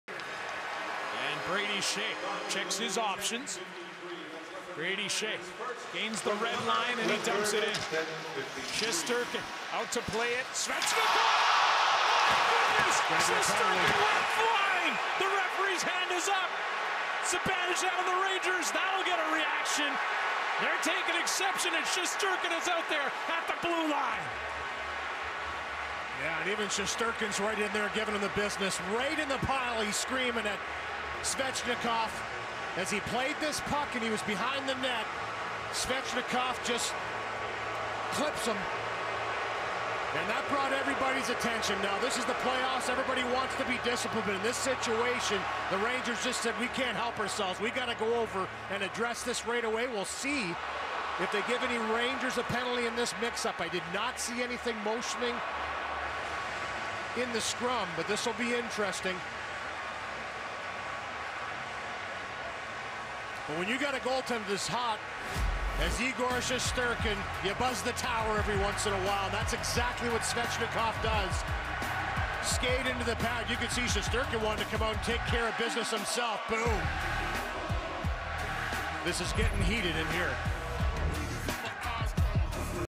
Andrei Svechnikov destroys Igor Shesterkin! Game 1 Rangers vs Hurricanes
Tribute video courtesy of the NHL and Sportsnet
Carolina Hurricanes vs New York Rangers Game 2